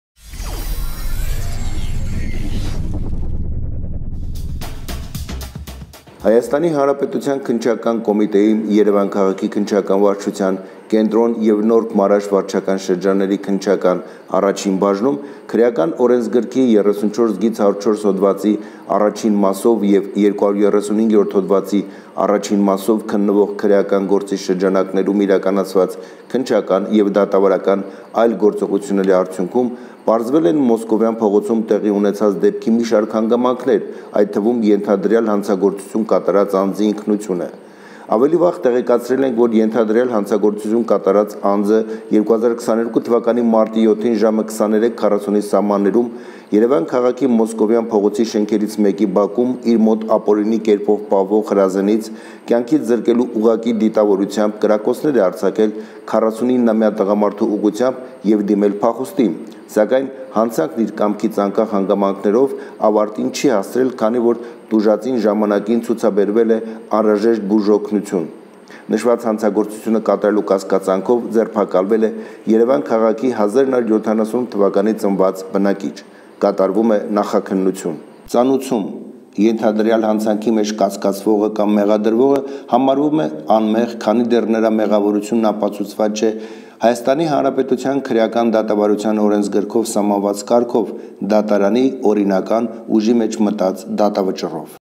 Կրակել էր դիմել փախուստի
Կրակել էր ու դիմել փախուստի․ 44-ամյա տղամարդը ձերբակալվեց